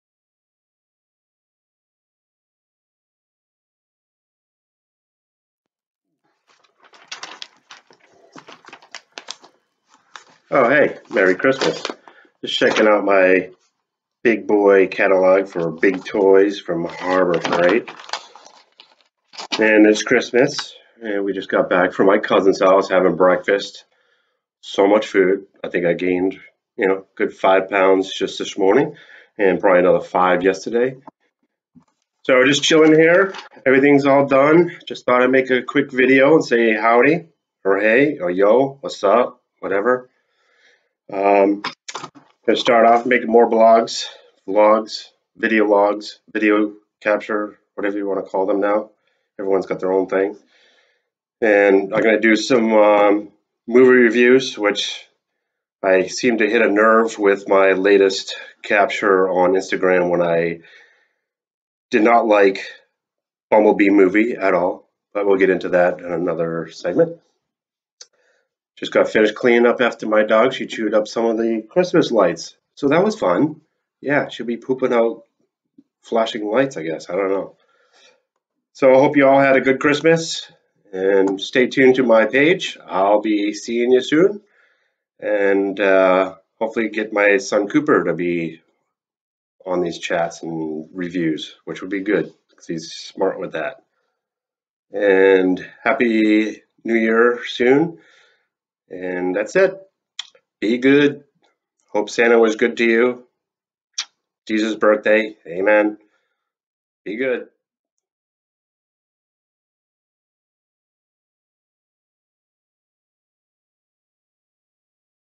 Merry Christmas vlog
A quick video to say Merry Christmas and i will be posting more videos